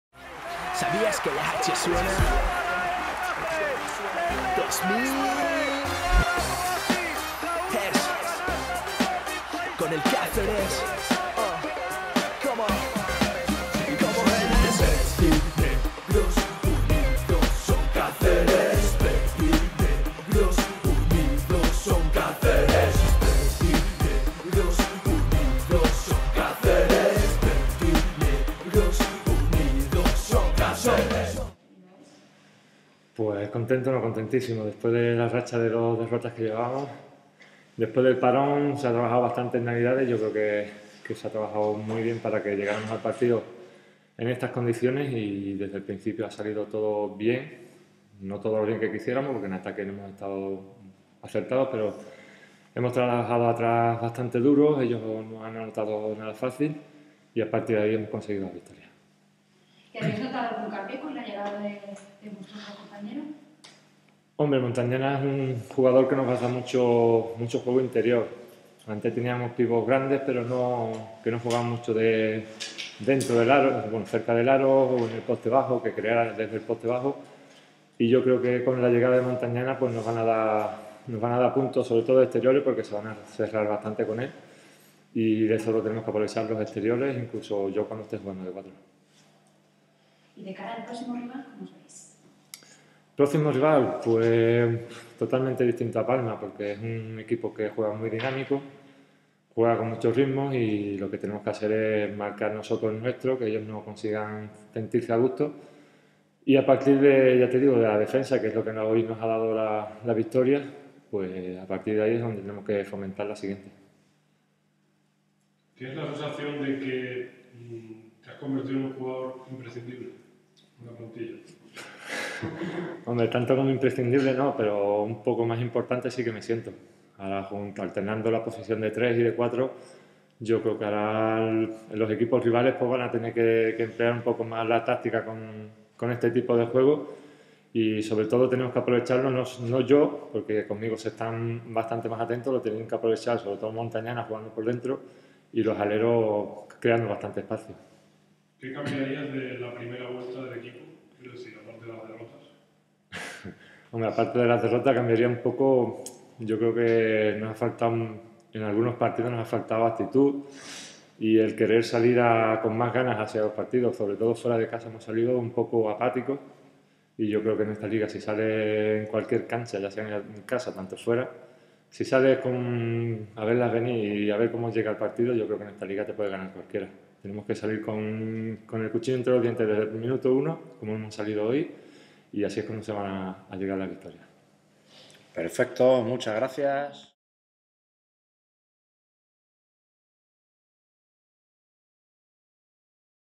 1314j13 juanmaruiz palma
Caceresbasket 2013/14 El jugador verdinegro, Juanma Ruiz, comparece ante los medios tras la victoria del Cáceres Patrimonio de la Humanidad (58-37) ante el Palma Air Europa en la jornada 13ª de la Adecco Plata